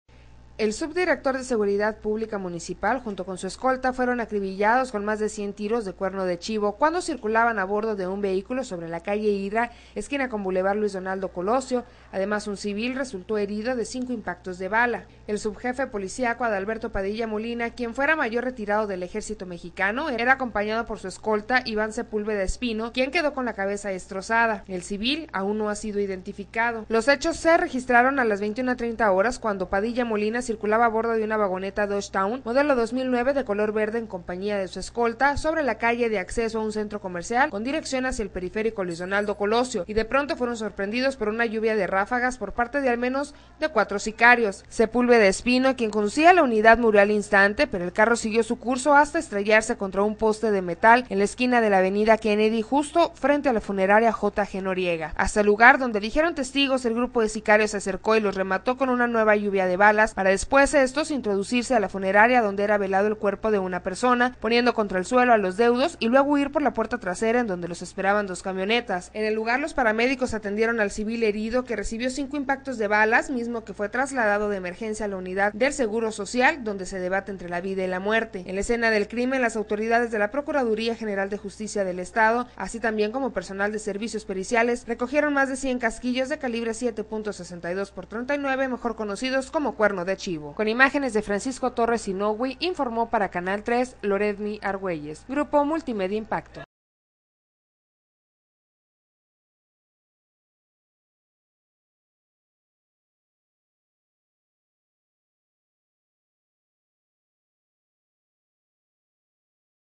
Muere subdirector seguridad publica
Nogales.- El subdirector de Seguridad Pública Municipal junto con su escolta fueron acribillados.